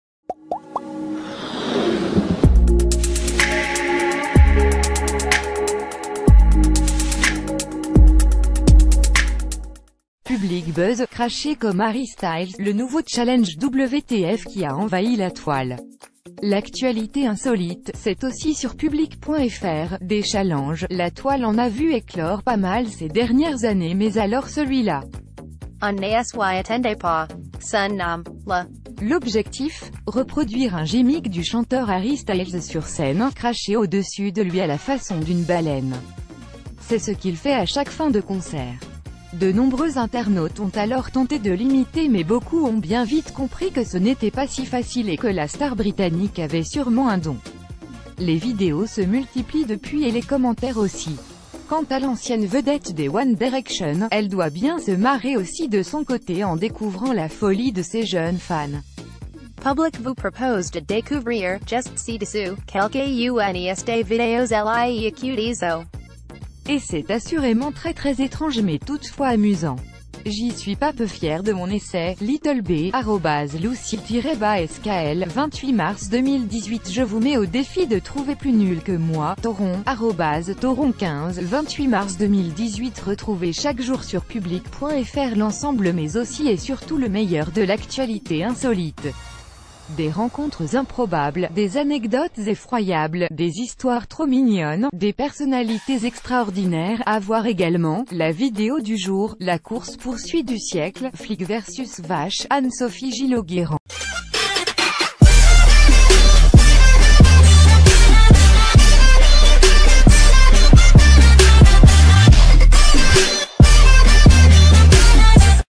Public Buzz : cracher comme Harry Styles, le nouveau challenge WTF qui a envahi la Toile !
Public Buzz : cracher comme Harry Styles, le nouveau challenge WTF qui a envahi la Toile ! . Public Buzz : cracher comme Harry Styles, le nouveau challenge WTF qui a envahi la Toile ! L'actualité insolite, c'est aussi sur Public.fr !Des challenges, la Toile en a vu éclore pas mal ces dernière... ...